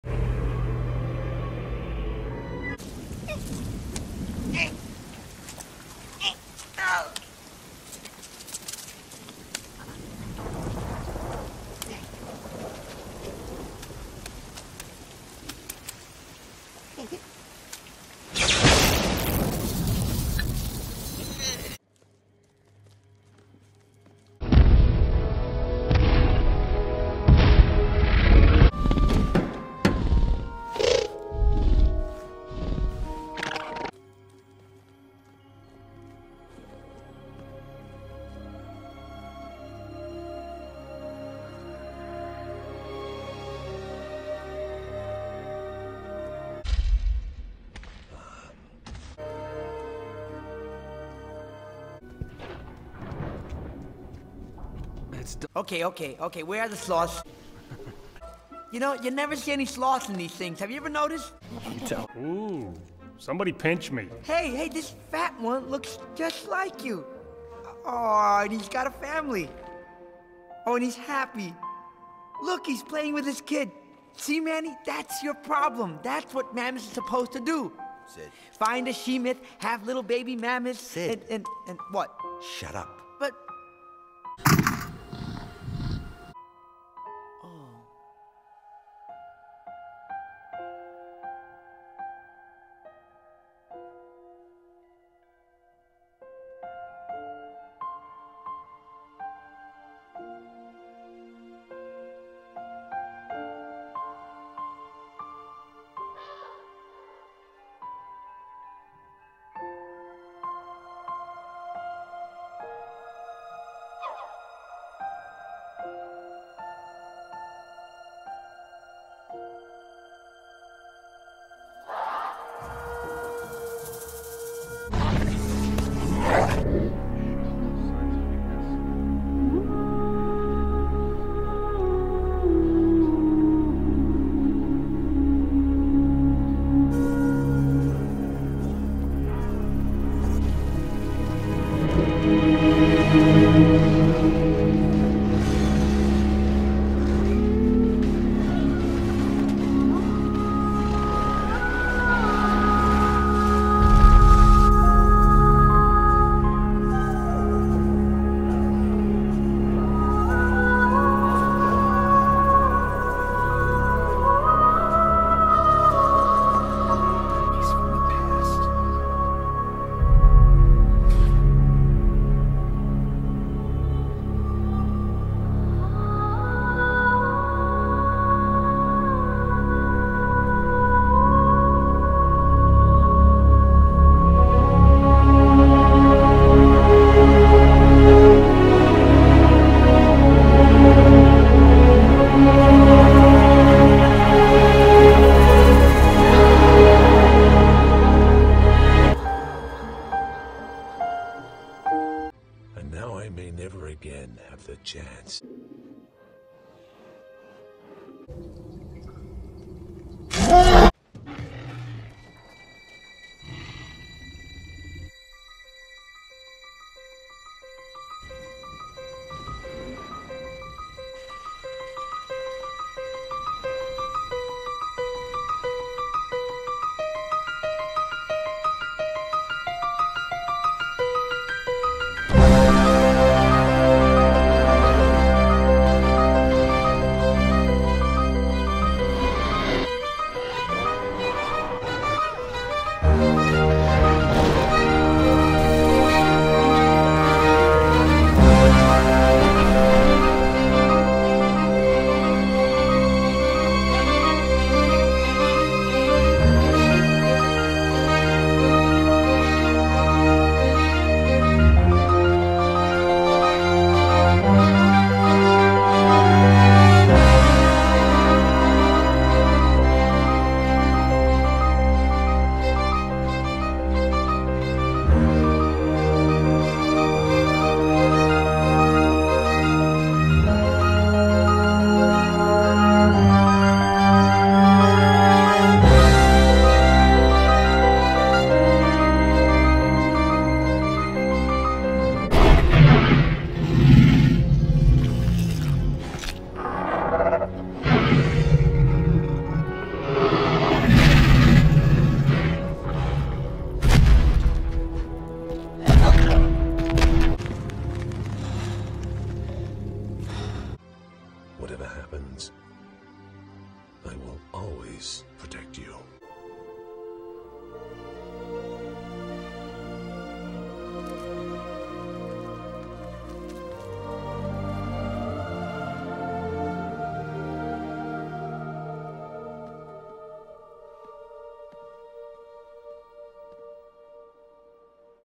Cave of Visions. Jurassic World meets Ice Age episode 4
Rexy, Manny, Blue, Sid, Speckles, Aladar and Diego enter a cave full of paintings and see visions of the past.